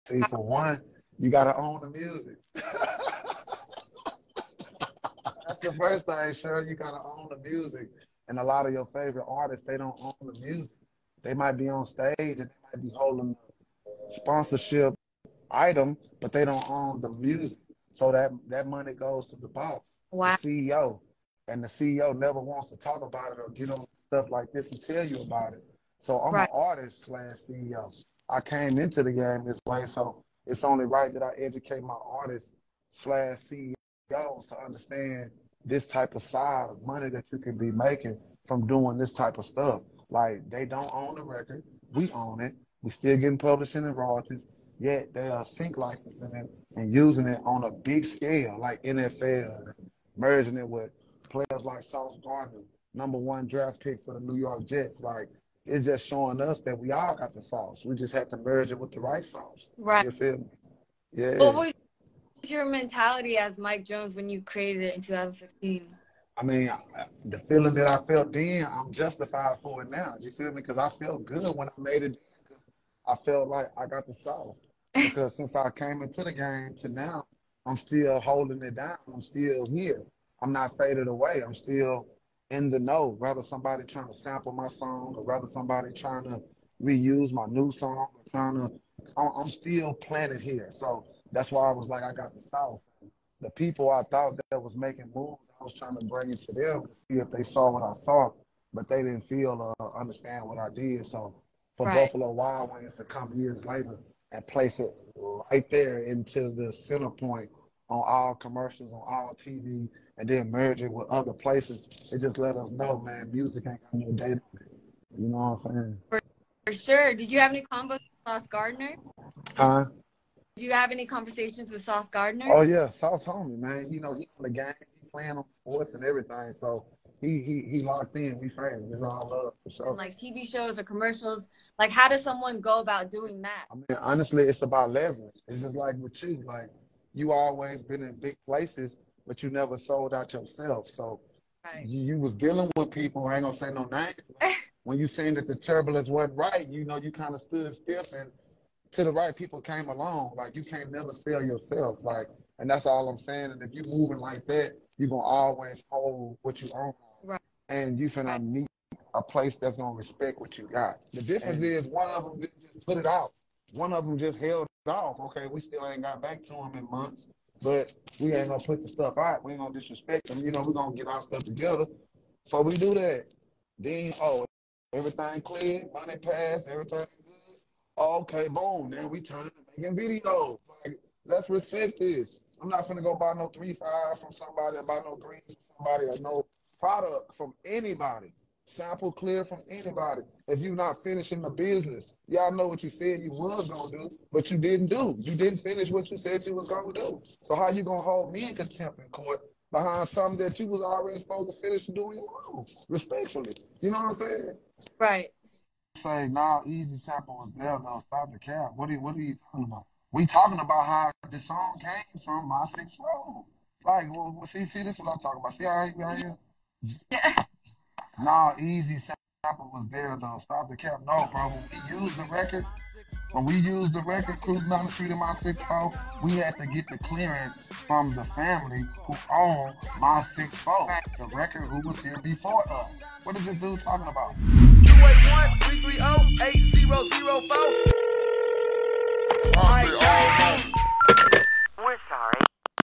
MIKE JONES X THE SOURCE INTERVIEW
MIKE JONES TALKS LICENSING & SAMPLING